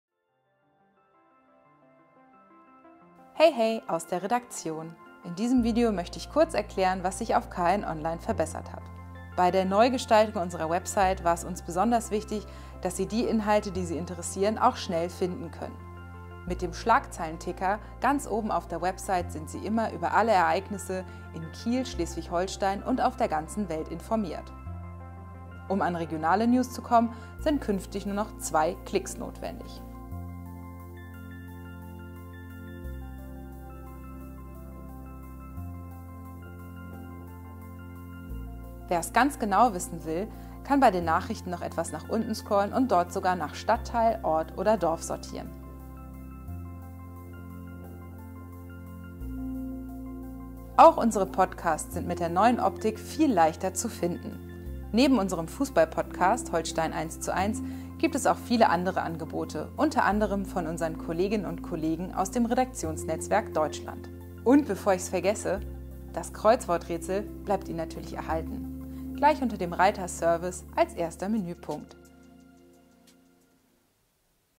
Schneller, besser, klarer: Was wir für Sie verändert haben
Neuanstrich für den Digitalauftritt: Auf KN-online.de erwarten Sie künftig
eine moderne Optik, schnellere Ladezeiten und viele weitere Funktionen.